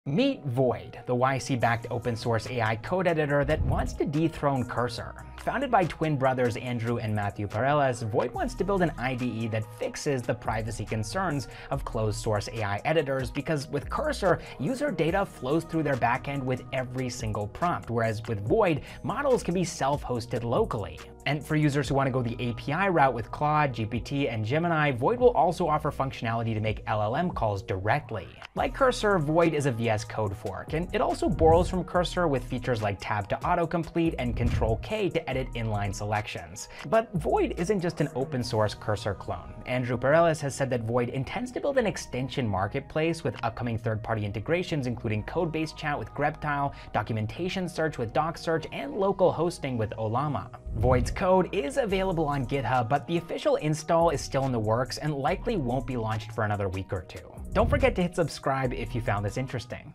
Is Void the Cursor Killer?
Cursor’s got competition. Meet Void, an open-source AI code editor that wants to dethrone Cursor AI.

Founded by twin brothers Andrew Pareles and Mathew Pareles, Void Editor aims to solve the privacy concerns inherent in closed-source AI editors. With Cursor AI, user data flows through Cursor’s back end with every prompt whereas with Void, models can be self-hosted using tools like Ollama.

For users who want to leverage models like Claude 3.5 Sonnet, ChatGPT, or Gemini via API, Void will also offer functionality to make LLM calls directly, rather than positioning themselves as a data middleman the way other AI editors have.

Like Cursor, Void Editor is a VS Code fork, and it also borrows from Cursor AI with features like Tab to autocomplete, Ctrl + K to edit inline selections, and Ctrl + L to chat. Even Void’s logo takes heavy inspiration from Cursor’s with a nearly pixel-perfect replication of its cuboid shape.

Void isn’t just an open-source Cursor clone though. Andrew Pareles has said that Void intends to build an extension marketplace. Upcoming third party integrations include codebase chat with Greptile, documentation search with DocSearch, and local hosting with Ollama.

When one of Cursor’s founders, Michael Truell, was asked why Cursor AI is an IDE instead of a plugin, he noted that Cursor’s Command + K and autocomplete features wouldn’t work if Cursor AI was a VS Code extension.

Andrew Pareles offered a similar response on why Void Editor forked VS Code instead of building a VS Code extension, highlighting the fact that VS Code extensions are often buggy or struggle to integrate naturally.

The previous startup of Andrew Pareles and Mathew Pareles, DeriveIt, is a NeetCode competitor that helps job seekers prepare for FAANG interviews. They built the entire platform from scratch and grew it to thousands of users before shifting their focus to Glass Devtools in 2024. The pivot Andrew Pareles and Mathew Pareles made from Glass to Void was likely spurred by Y Combinator. It’s also worth noting that another S24 Y Combinator startup, Melty, is building an open-source AI code editor too.

Void’s code is available on GitHub. The official install is still in the works though and likely won’t be launched for another week or two.